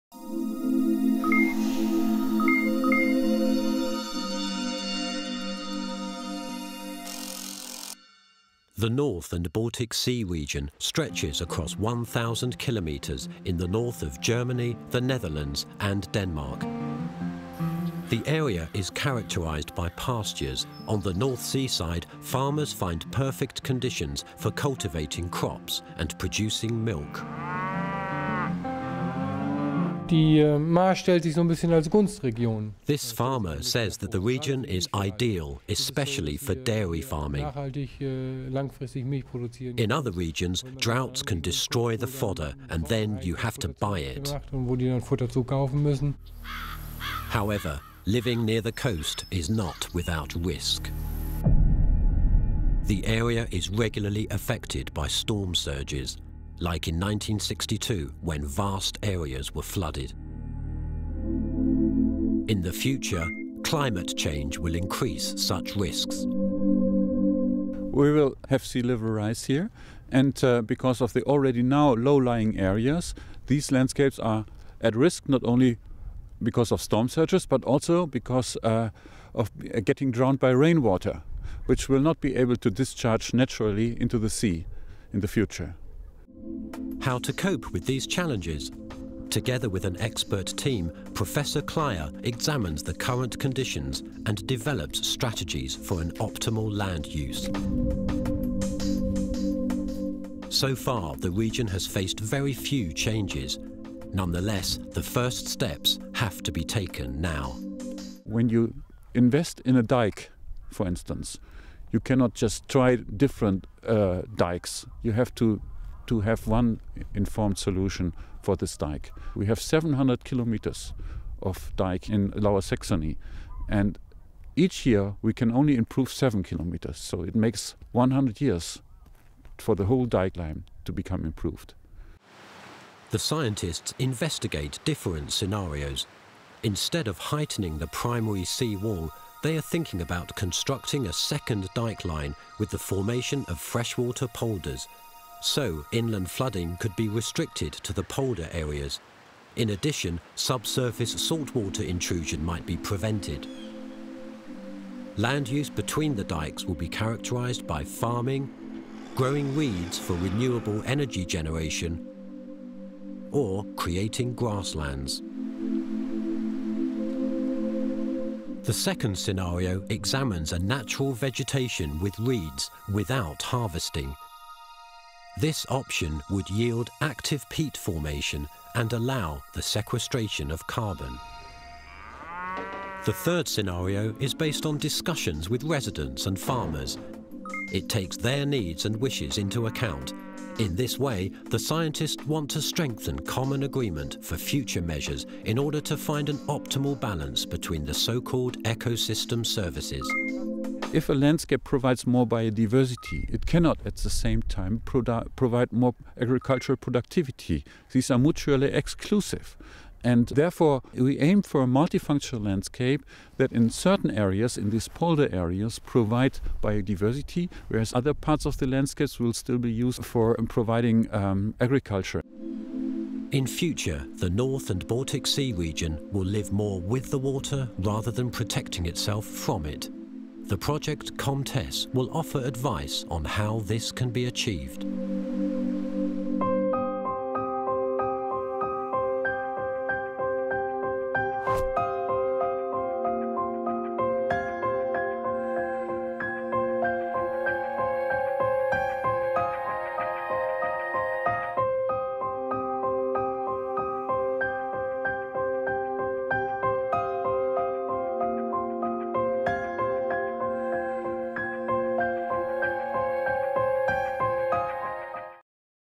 COMTESS - Sustainable coastal land management: Trade-offs in ecosystem services
Development of a sustainable coastal zone management for the Geman coast under climate change and sea level rise.